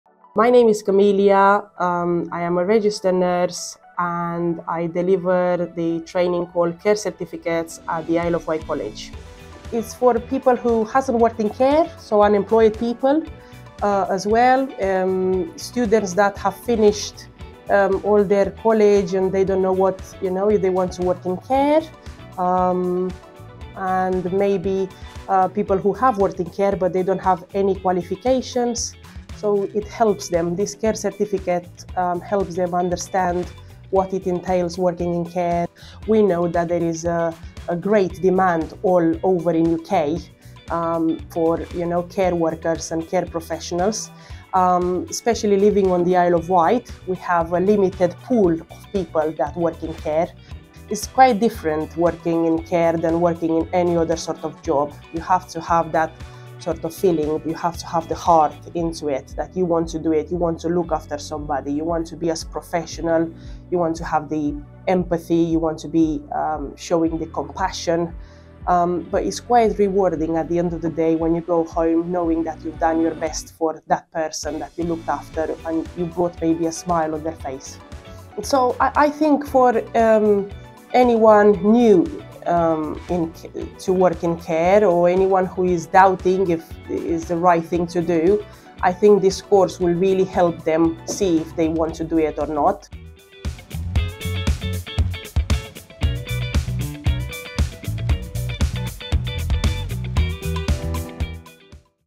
Camelia Moise Gogoasa - Solent Partners' Health & Social Care Skills Bootcamp, Isle of Wight College
Nurse Camelia Moise Gogoasa, delivers the Health and Social Care Skills Bootcamp at the Isle of Wight College, said: “This course is essential for those unemployed, anyone new to the care industry or those looking to enhance their current skills.

It offers valuable mentorship from experienced professionals who are eager to share their knowledge and expertise."

“For employers, this course is an excellent opportunity to recruit and train more healthcare workers, ensuring staff are well-equipped to progress within the company and fill vacant posts. Plus, for existing employees, it’s a fantastic way to enhance their current skillset in order to move up the career ladder. By participating, employers can help shape and influence care-related skills bootcamps to meet the growing demands of the industry.”

“The Isle of Wight College Bootcamp is perfect if you prefer hands-on learning with a mix of classroom and online sessions, plus it comes with excellent career support,” said Camelia,